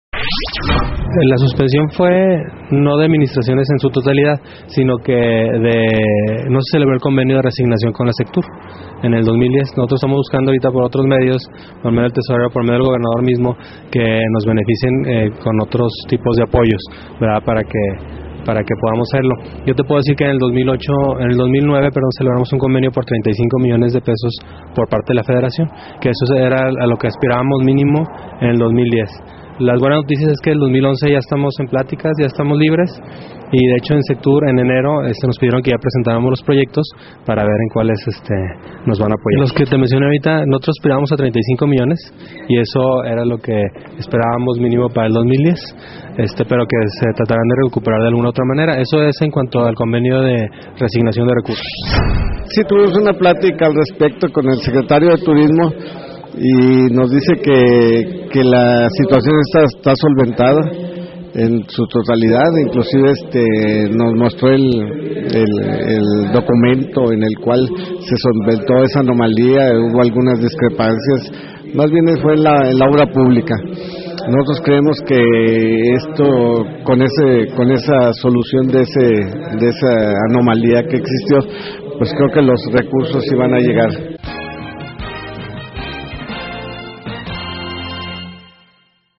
Buscan restituir el daño presupuestal de 35 mdp.
El secretario de Turismo del estado, Juan Ávalos Méndez, aseguró que buscan restituir el daño presupuestal de 35 millones de pesos en ministraciones que se suspendieron por parte de la Secretaría de la Función Pública (SFP). El líder de los hoteleros en Durango, Francisco Martínez Díaz de León confirmó que se reunieron con autoridades turísticas en la entidad para solucionar esta merma presupuestal.

VIN/URI/10/12/10